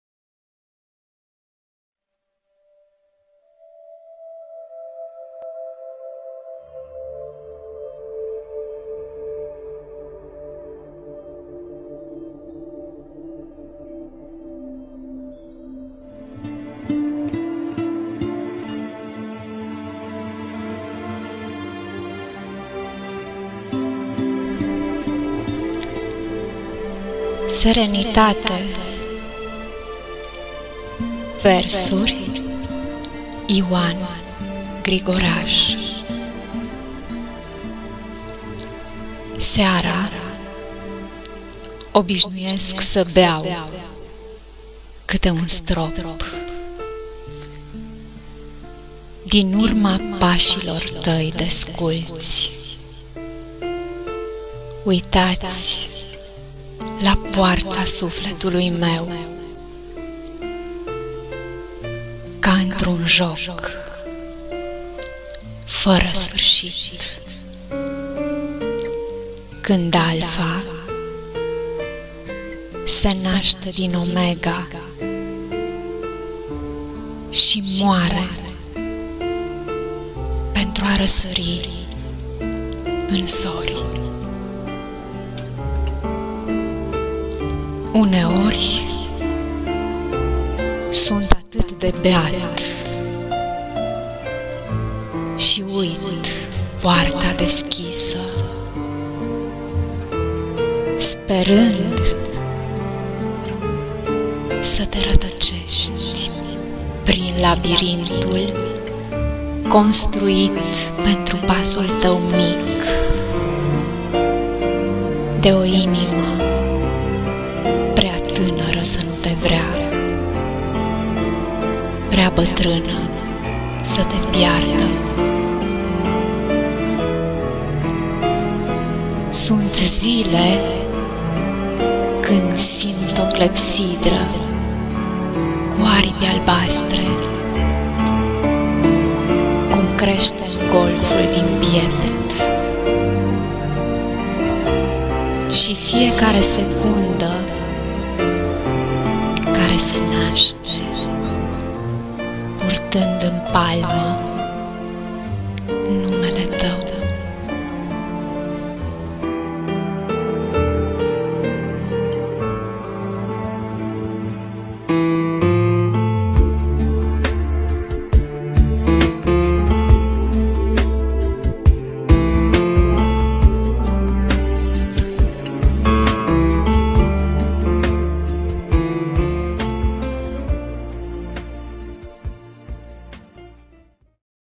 Serenitate Versuri : Ioan Grigoraș
seara
obișnuiesc să beau
câte-un strop
din urma pașilor tăi desculți
uitați la poarta sufletului meu
ca într-un joc fără sfârșit
când alfa se naște din omega
și moare pentru a răsări în zori

uneori
sunt atât de beat
și uit poarta deschisă
sperând să te rătăcești
prin labirintul construit pentru pasul tău mic
de o inimă prea tânără să nu te vrea
prea bătrână să te piardă

sunt zile
când simt o clepsidră
cu aripi albastre
cum crește în golful din piept
și fiecare secundă
care se naște
purtând în palmă numele tău